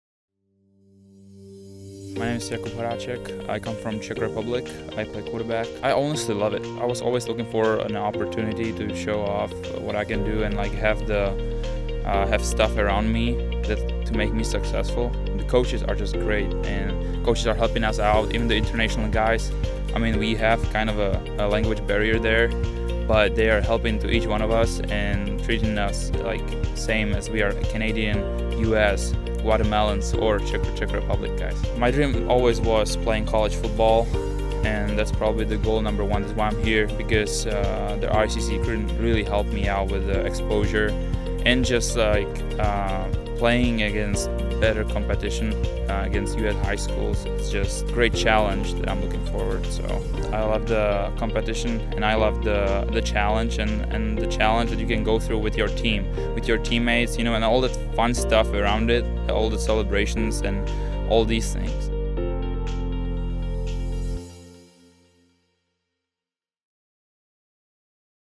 Meet RICC Captain Jakub Horacek
Captain Jakub Horacek '18 is a quarterback from Brno, Czech Republic. Find out what Jakub thinks of his Knights football team so far.
Royal Imperial Knights